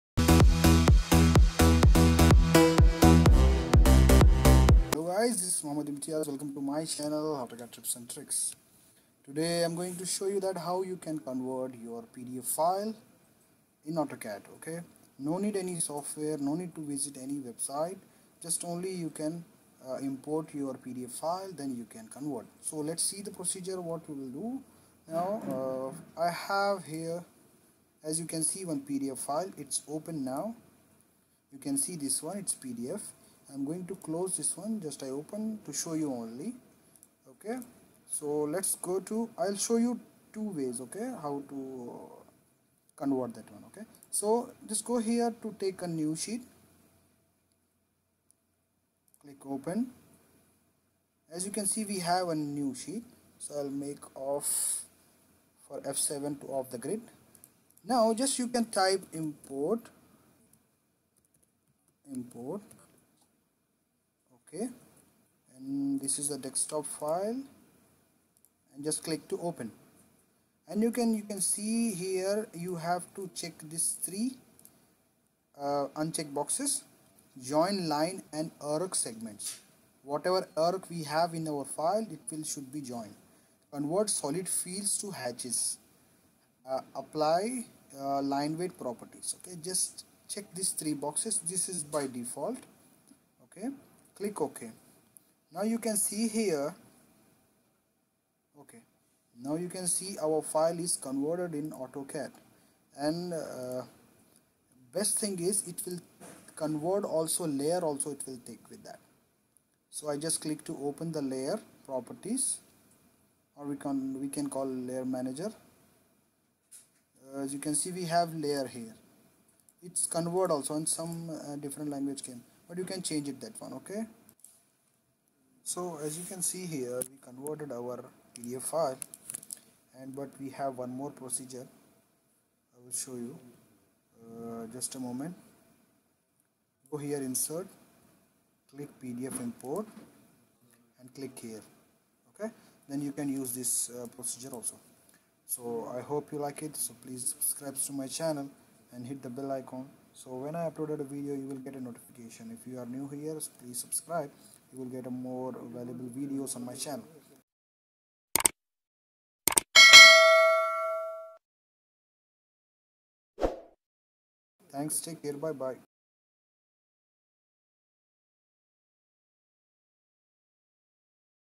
How to convert PDF to AutoCAD in just 2 minutes 😲
Here you can learn how to convert PDF to AutoCAD by just importing the PDF file it will take just 2 minutes and your file will be convert in AutoCAD so we don't need any other software or not required to visit any website it's only use your AutoCAD to covert

You can see very surprising tutorials 😉